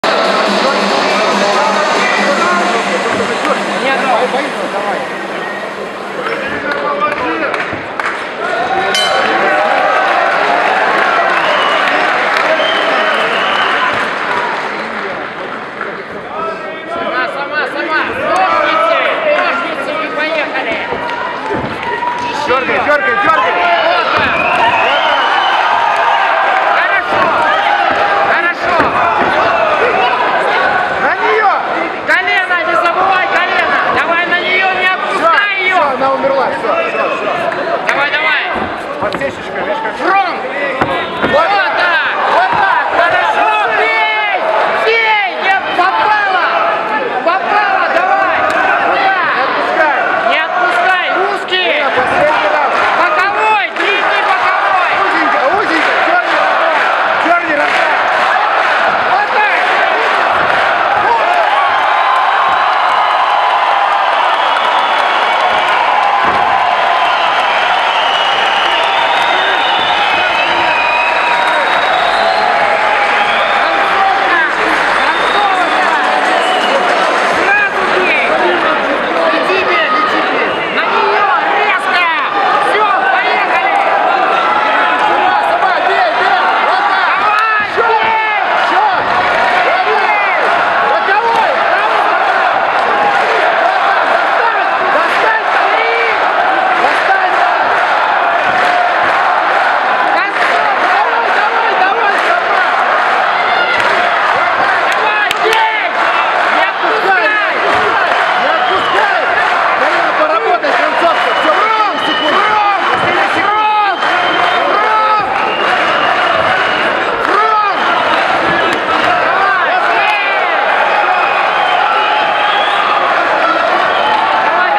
Liepājas kikboksere Karīna Jarjomenko uzvar Indijas sportisti
Liepājas Olimpiskajā sporta centrā pasākuma "Boksa vakars ar Mairi Briedi" laikā notika Eiropas populārākā boksa komercturnīra "Bigger’s Better" organizatoru pilnīgi jauns produkts – “Bigger’s Better Girl Power” kikboksa turnīrs. Tajā varonīgi cīnījās arī liepājniece Karīna Jarjomenko. Viņa pirmajā kārtā pārspēja Indijas sportisti.